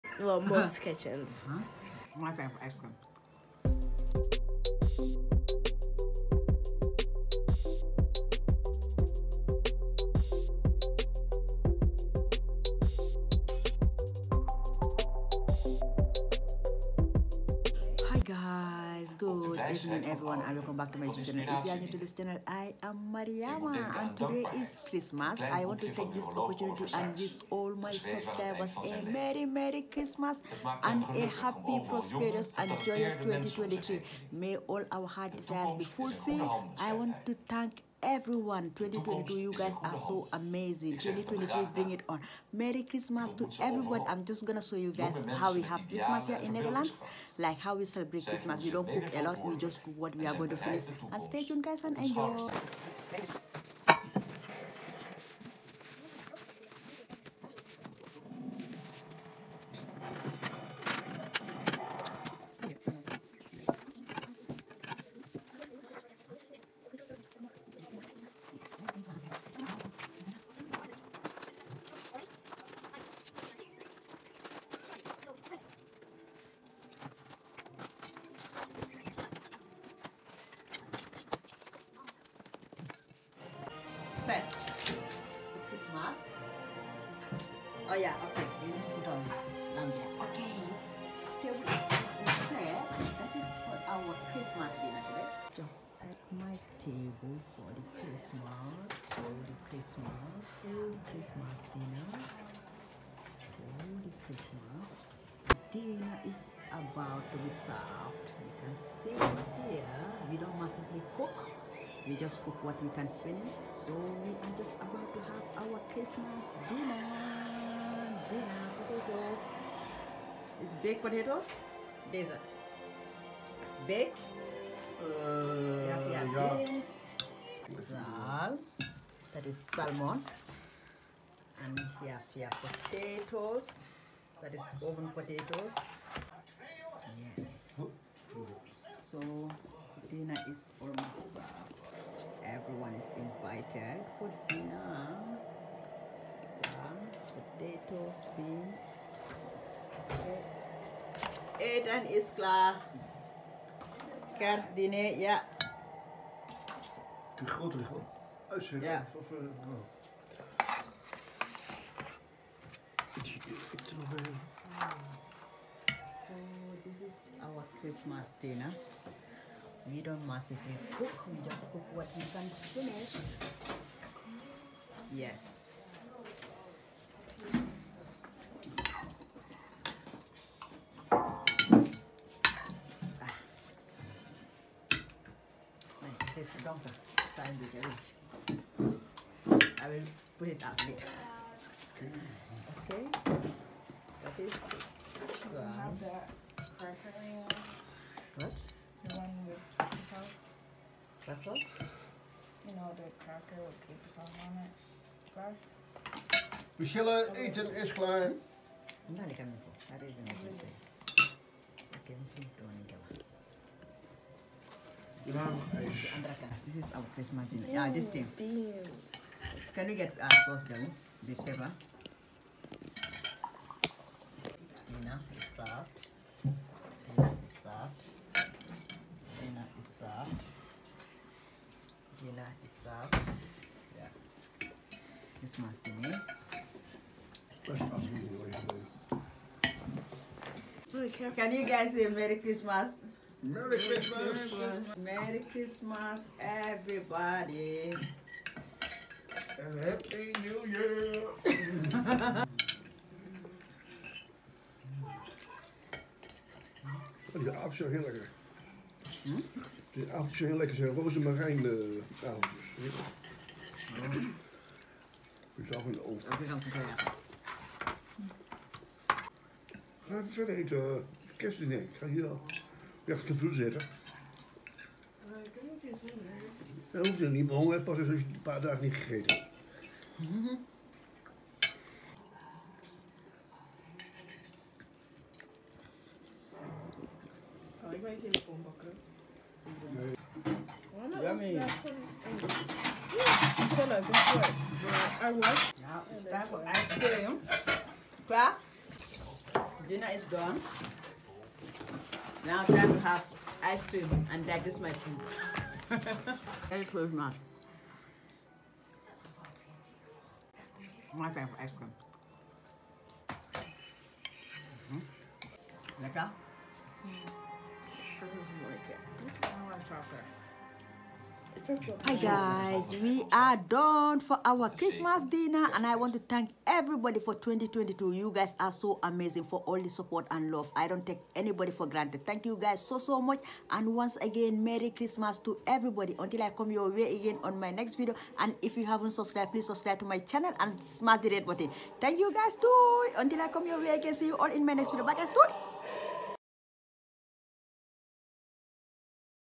Christmas DINNER with us|Merry Christmas to all
In this video we had a lovely and delicious xmas diner and it was so so amazing.

We don't massively cook and we cook what we can finish.Christmas is so nice and everyone had a delicious food and later we ate ice cream as well.

Wishing everyone a merry and joyous new year in advance with lots of blessings and good health.

Merry Xmas to all once again.